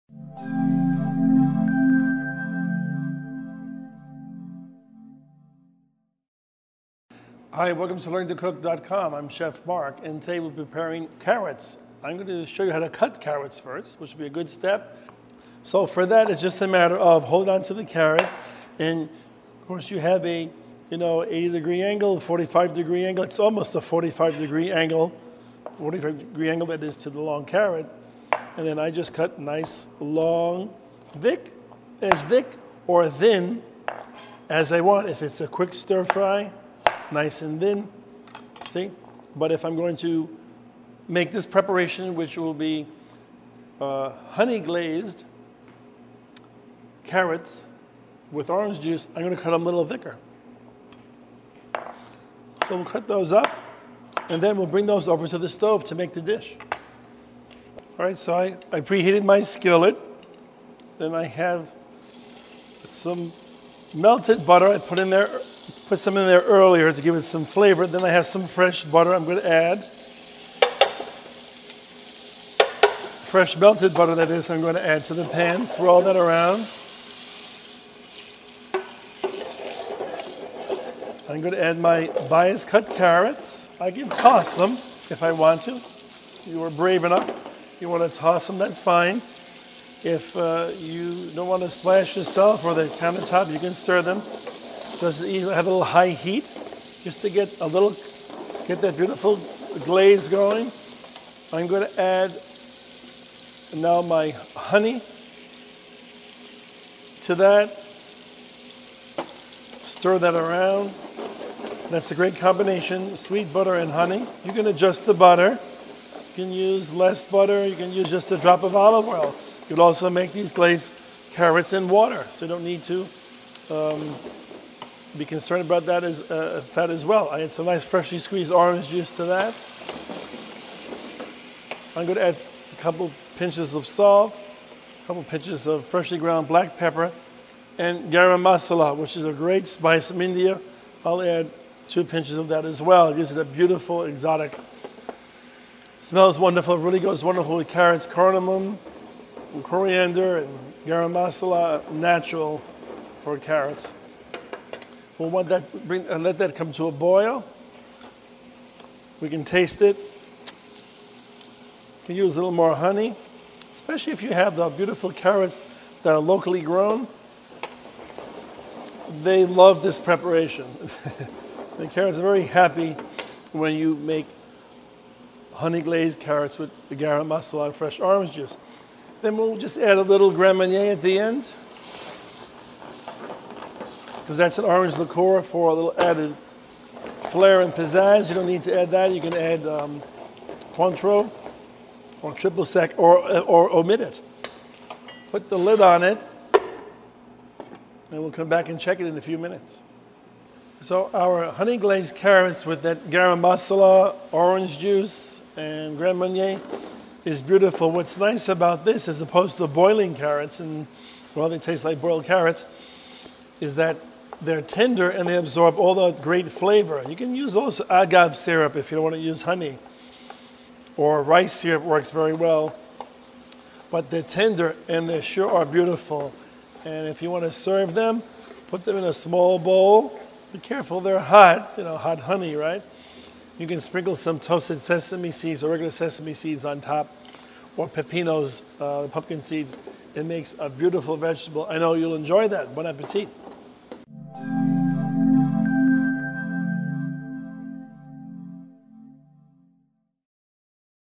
How to Cook Honey Glazed Carrots
Sweet, salty and sometimes even spicy, glazed carrots make a complimentary side for anything you're serving up.

Ingredients:
1 pound carrots
1 ounce whole butter
salt and pepper  to taste
1 fluid ounce maple syrup
1/2 tablespoon fresh parsley – chopped

Method
1. Peel the carrots and cut into a shape such as an oval, square, oblique, tournée or round thick slice.
2. Parboil the carrots in salt water and drain. The carrots should be very firm.
3. Sauté the carrots in butter until nearly tender.
4. Season with salt and pepper, and add the maple syrup. Cook briefly, tossing the carrots so that they are coated with the maple syrup. Garnish with the parsley.